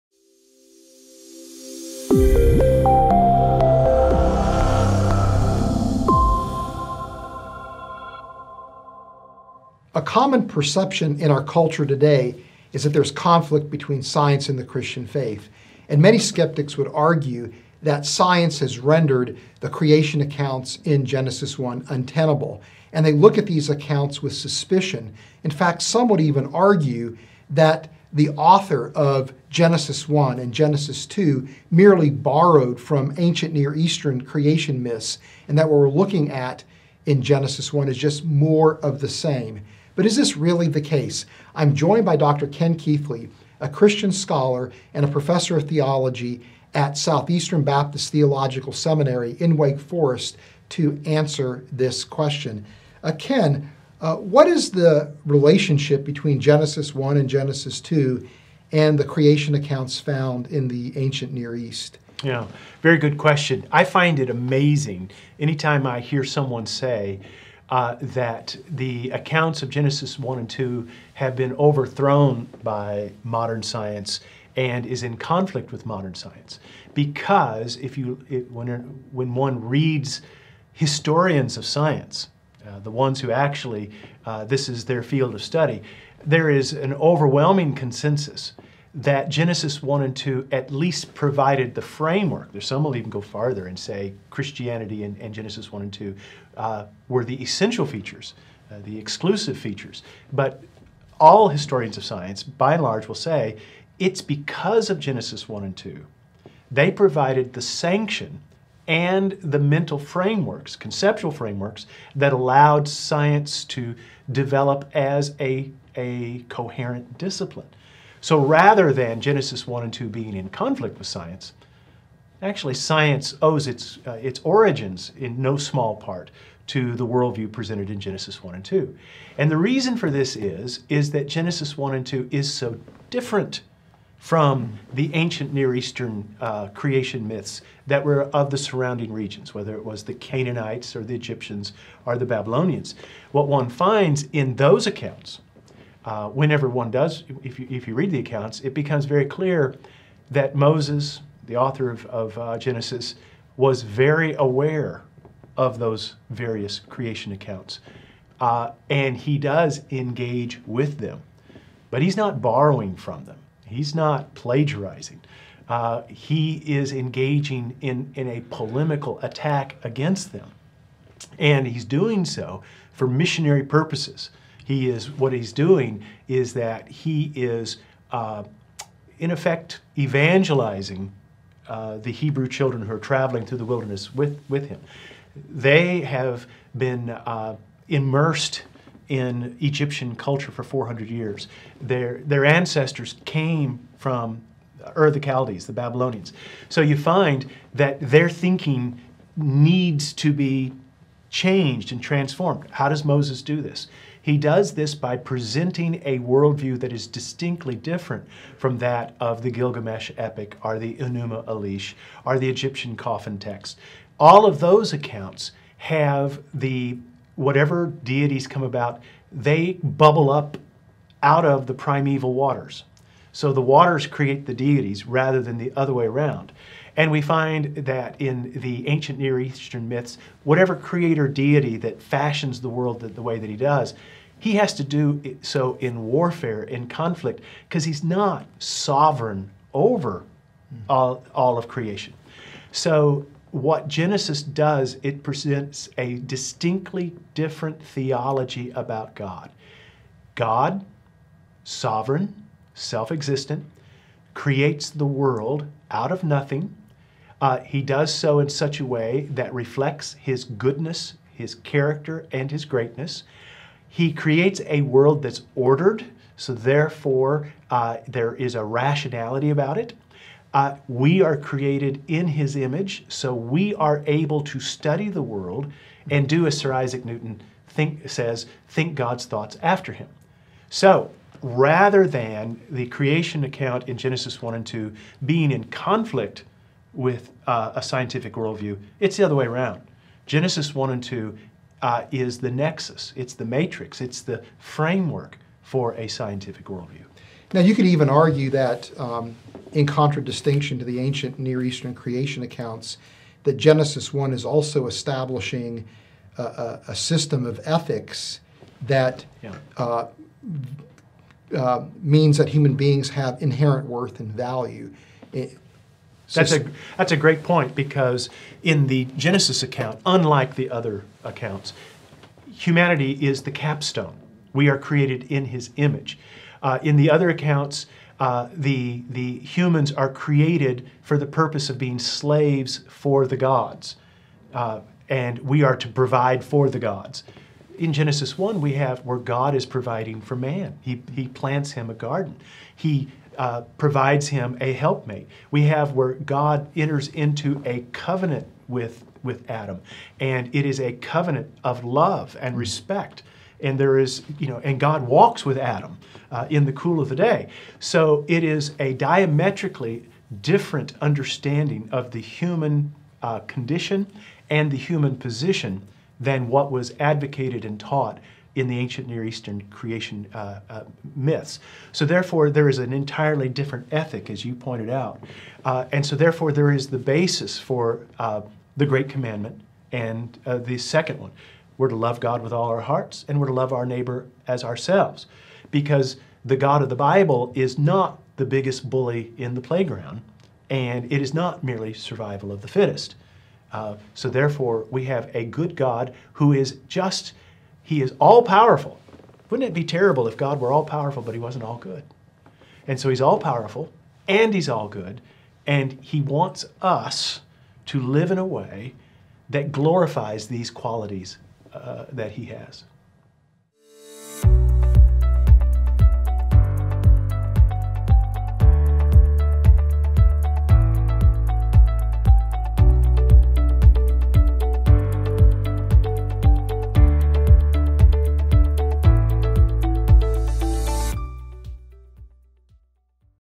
Genesis Creation Accounts
Ken Keathley is a senior professor of theology and occupies the Jesse Hendley Endowed Chair of Biblical Theology at Southeastern Baptist Theological Seminary in Wake Forest, North Carolina, where he has been teaching since 2006. He also directs the L. Russ Bush Center for Faith and Culture, a redemptive influence that seeks to engage culture, present and defend the Christian faith, and explore its implications for all areas of life. He is the coauthor of 40 Questions about Creation and Evolution (Kregel, November 2014) and coeditor of Old Earth or Evolutionary Creation? Discussing Origins with Reasons to Believe and BioLogos (IVP, July 2017).